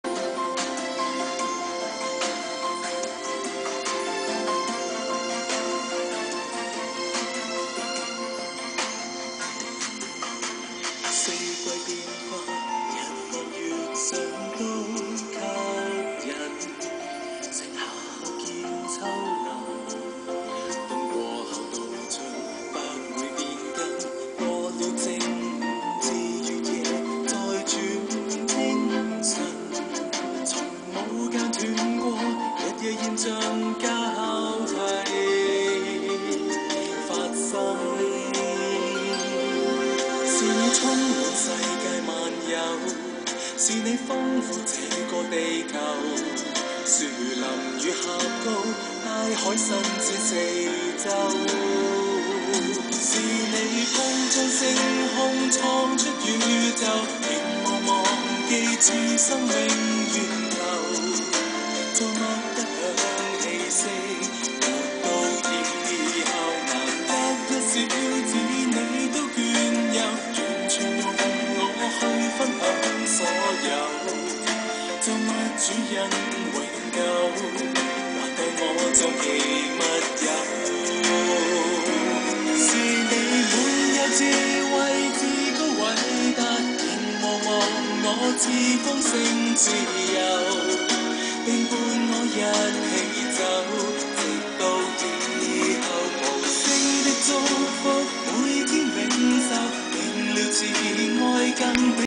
Cute Kitten and his computer
Meow Meow is always accompanying me when using my computer.